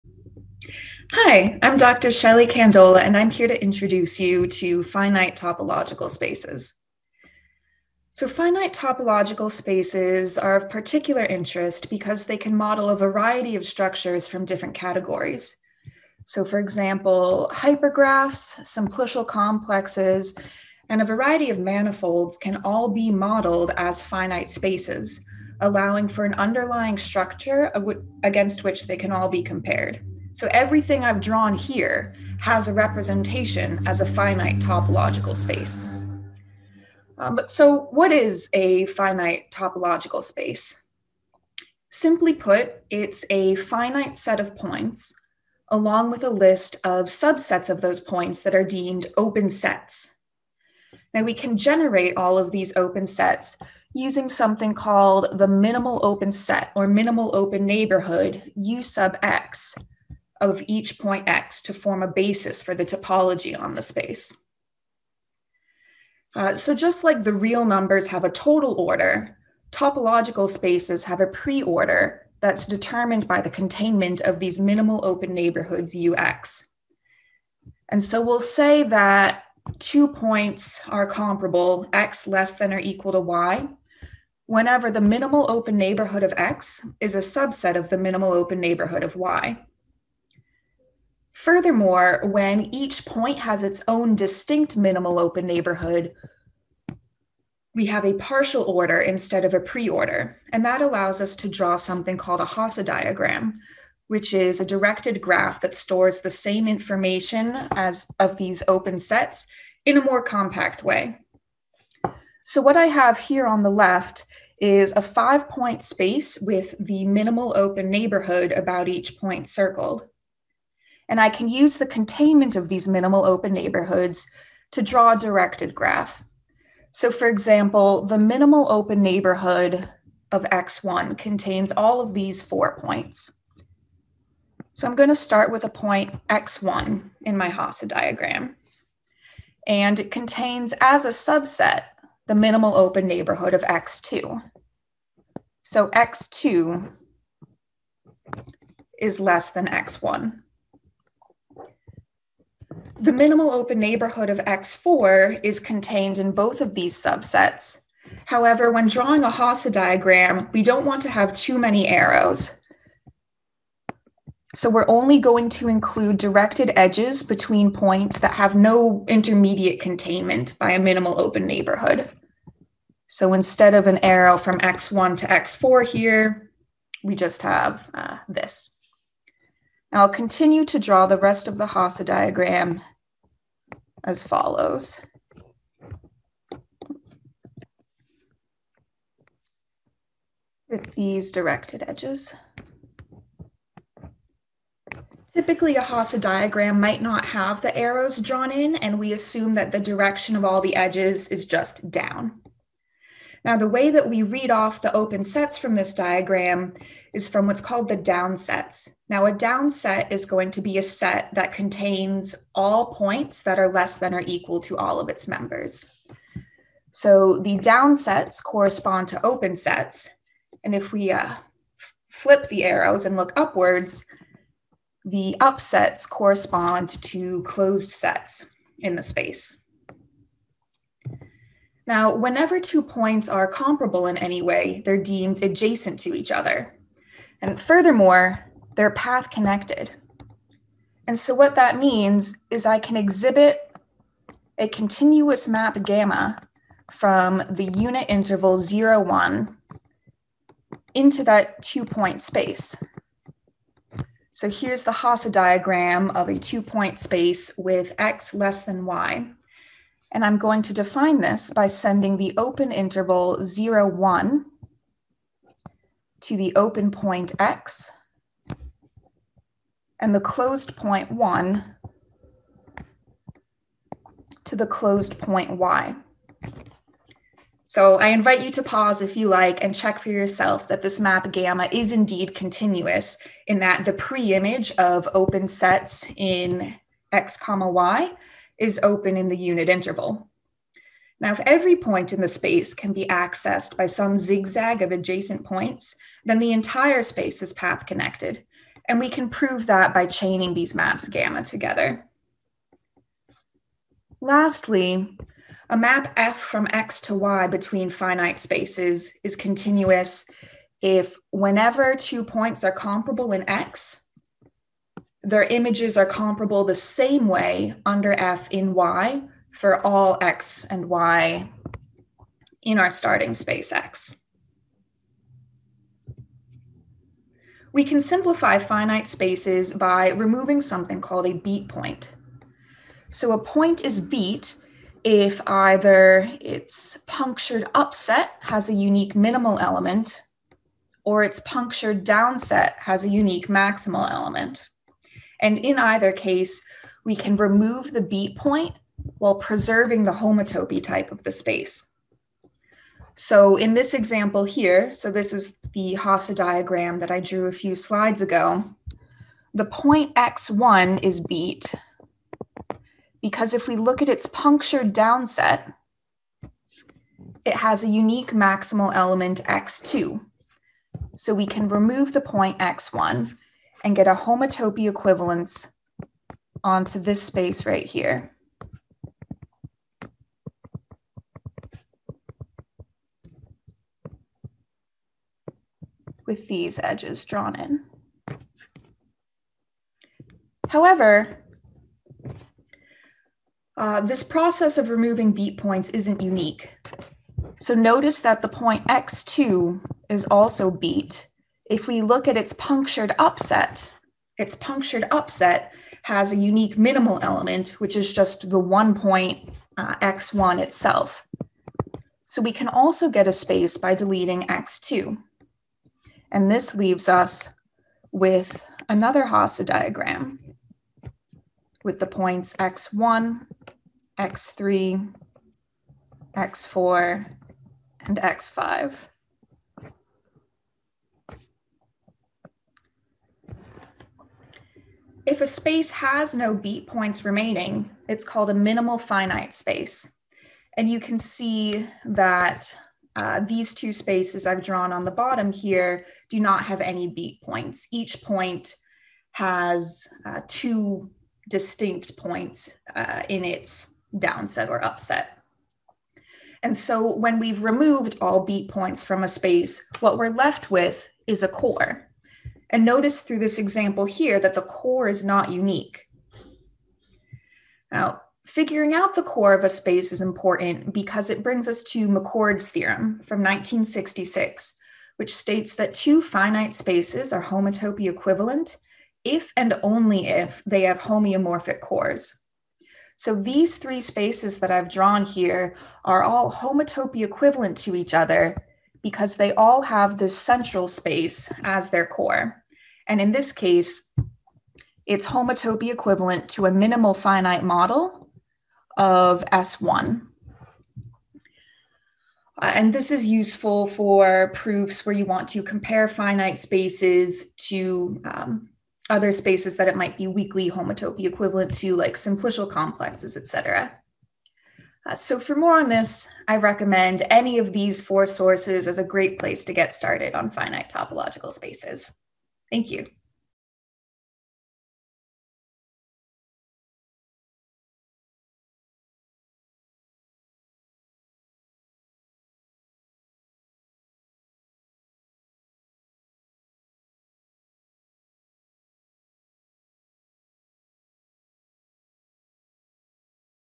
Finite Topology [Shelley Kandola]
In this tutorial, I introduce the basics of finite topological spaces. We learn about open sets, path-connectivity, and continuous maps. We also learn how to delete points from a finite space while preserving the homotopy type. This leads to our conclusion, a statement of McCord's Theorem for classifying the homotopy types of finite spaces.

This tutorial was contributed as part of the WinCompTop+AATRN Tutorial-a-thon in Spring 2021